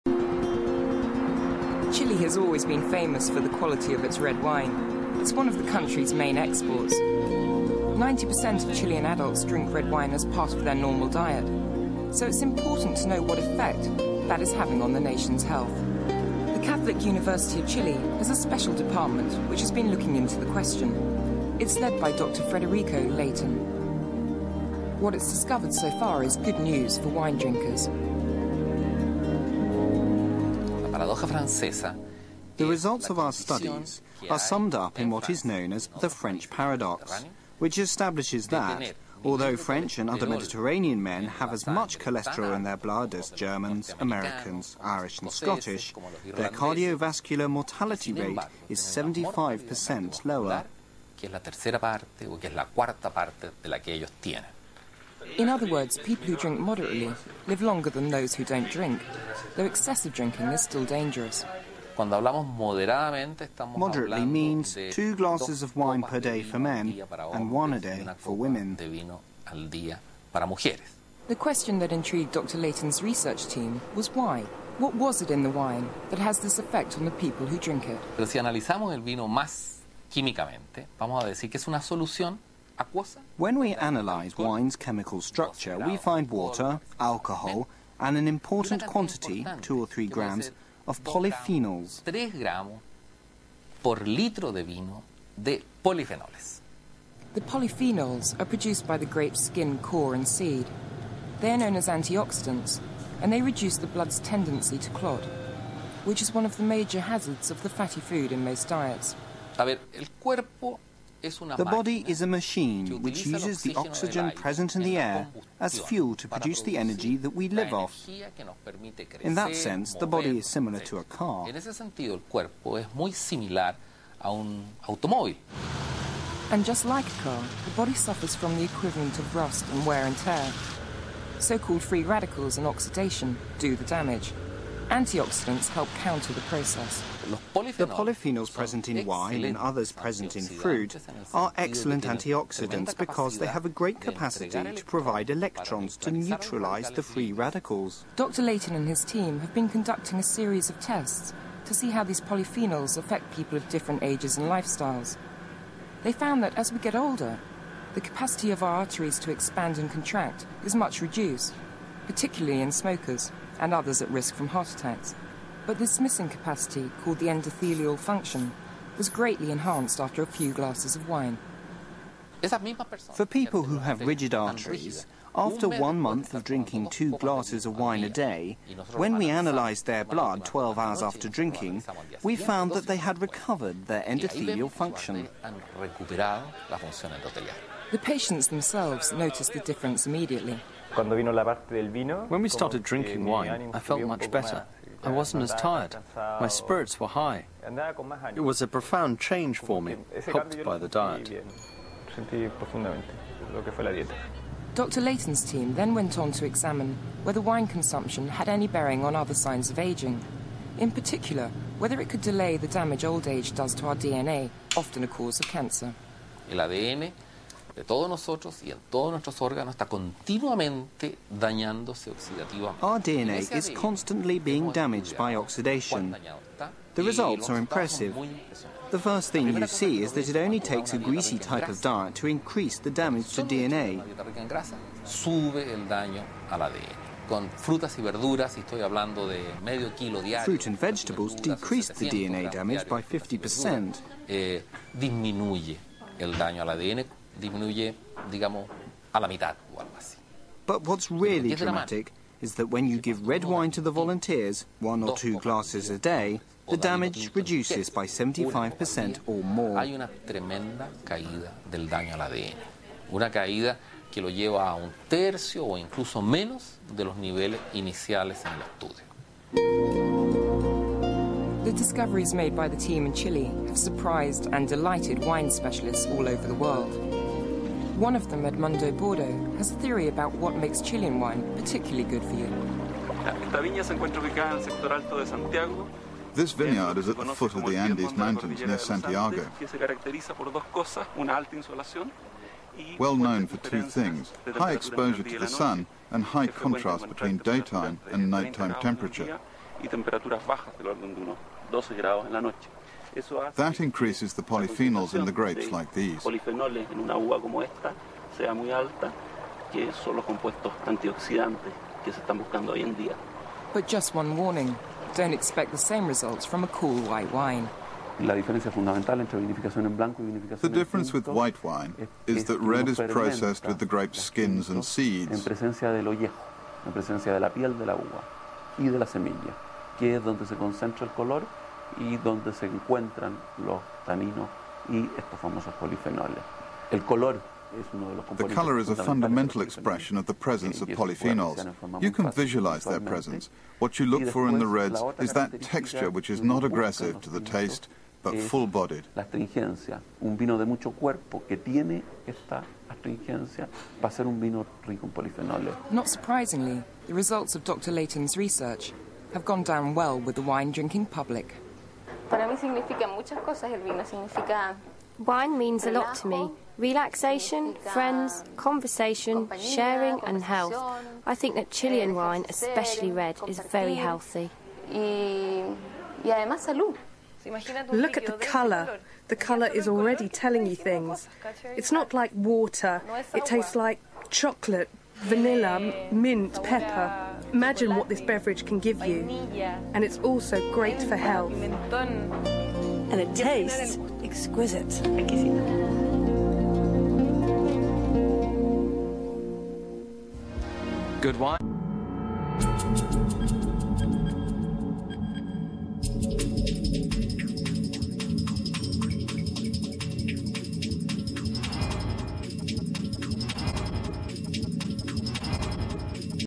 Ageing Wine - Chile
18 October 2001
Researchers in Chile have unearthed the secrets of red wine -- now there's scientific proof that a little of what you fancy does you good.